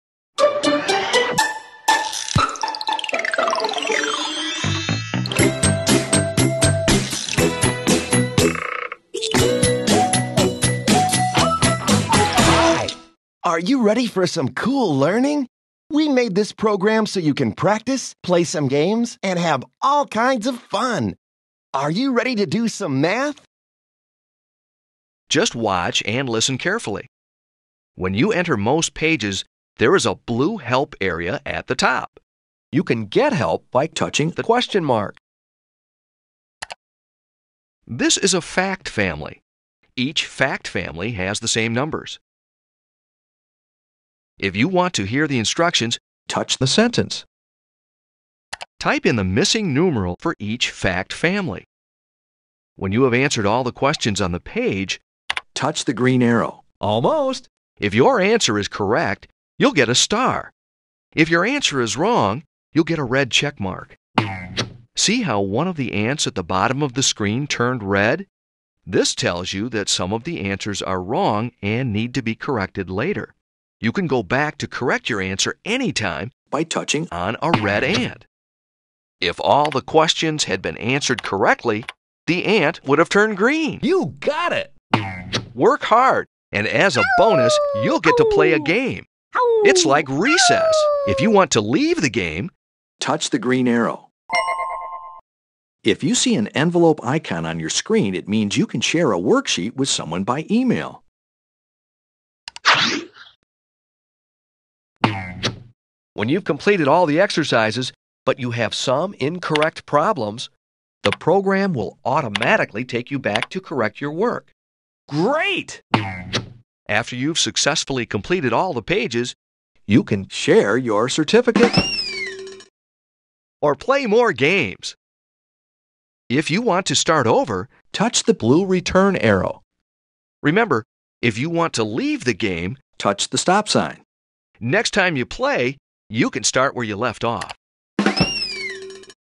On-Track Math 2
Give your second grader a math boost. Mastery of core skills means moving on to new challenges with ease and confidence. Making it fun is a big bonus!

With the On-Track Math 2 program kids can practice counting by 2s, 5s, and 10s, adding and subtracting two- and three-digit numbers, sequencing numbers, working with place value, arranging object sets, multiplying, and more. This app "tracks" a child's progress, color-coding correct/incorrect answers and giving them an at-a-glance review of their work. If they make a mistake they can select the red ants at the bottom of the screen and rework the problem. Combined with audio guidance, step-by-step instructions, and instant grading, Math 2 promotes self-paced learning. Colorful graphics, silly sound effects, and funny reward animations enhance the learning experience. A little "recess" is also built in! At intervals in the exercises, kids are rewarded with three alternating games: Snake Racer, Kangaroo Obstacle Course, and Howling Coyotes. Plus, a progress report can be printed at any time, as well as bonus exercise pages and a completion award certificate when finished.

Features
•  Audio guidance
•  Visual examples
•  Instant grading
•  Progress-tracking
•  Positive reinforcement
•  Bookmarking
•  Reward animations
•  Three fun games
•  Randomized problems
•  Early math terminology
•  Printable report and certificate

Skills: multiplication; factors and products; facts and fact families; two- and three-digit addition and subtraction; place value; greater than/less than; numbers through 999; ones, tens and hundreds; sequencing; skip counting by 2s, 5s, and 10s

(U.S. English and math conventions)